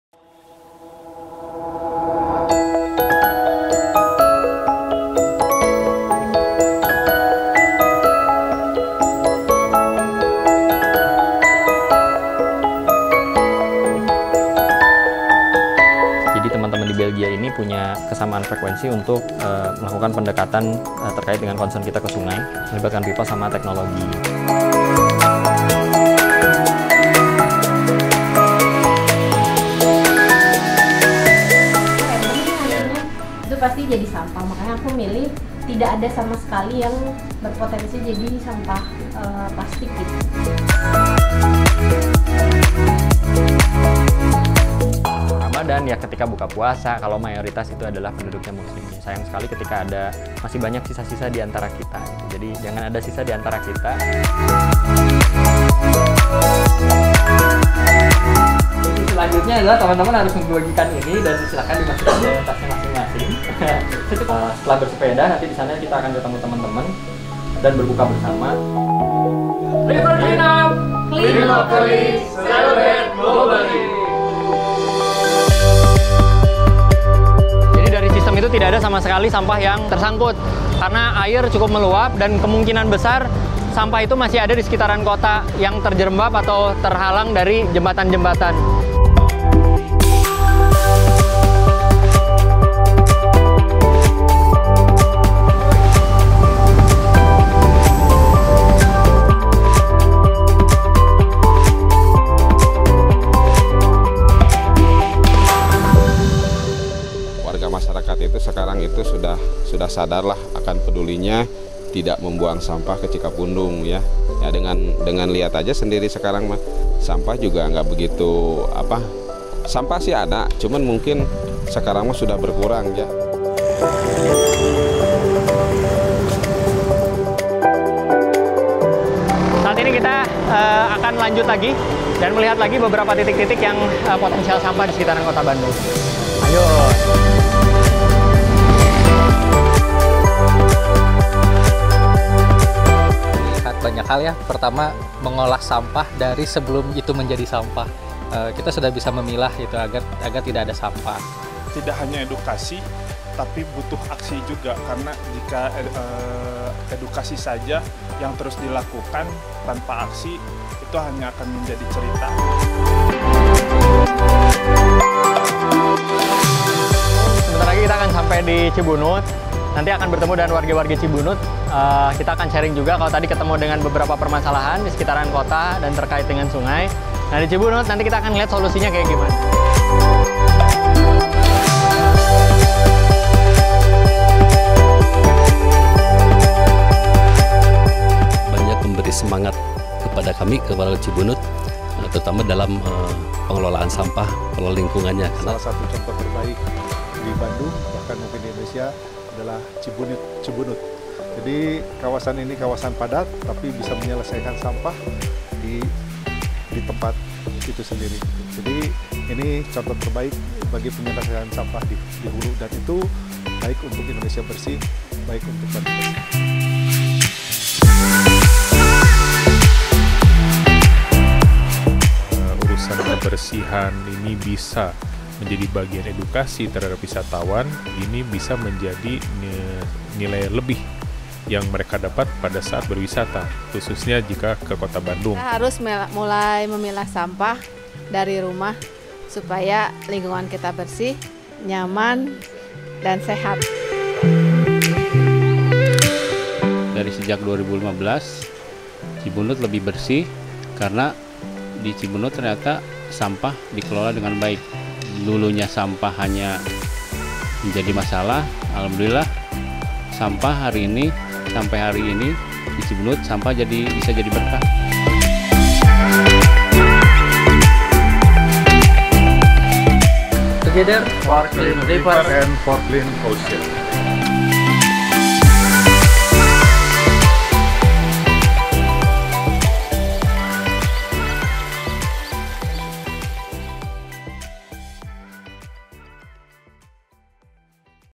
River Cleanup Indonesia: PEDAL4CLEANRIVER #1
PEDAL4CLEANRIVER - Green Iftar Edition⁣
Sebagai rangkaian penutup kampanye ramadanlessplastic kami bersepeda sore hari menjelajahi kota Bandung dengan membawa misi khusus.⁣
Rekan-rekan yang bergabung terdiri dari bagian komunitas, edukator, asosiasi penggiat pariwisata, pemerintahan dengan pelibatan kelompok masyarakat kawasan urban dan social influencer yang sama-sama memiliki concern terhadap kondisi sungai juga permasalahan sumber sampah selama bulan ramadan.⁣
Ada 50 green iftar package yang kami kemas bersama-sama, takjil ramah lingkungan tanpa menghasilkan sampah dengan kemasan yang bisa dipakai ulang, tur bersepeda menjelajah sisi kota yang tidak biasanya menjadi metode edutainment yang kami gunakan kali ini.⁣
Sudah siap mengayuh sepedamu kembali selepas ramadan?⁣